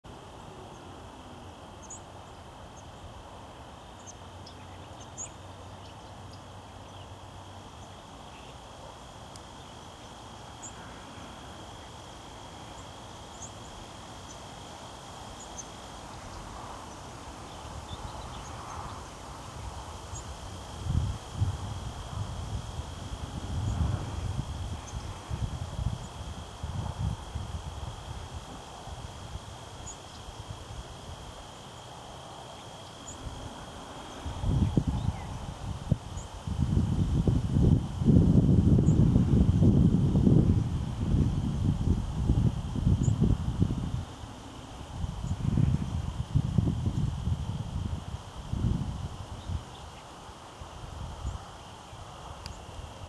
Immature North American White-crowned Sparrow on bush
An immature White-crowned Sparrow with the brown head instead of the black and white of an adult moves about on a bush in a prairie in the northern USA.
where videos are straight from the camera unedited.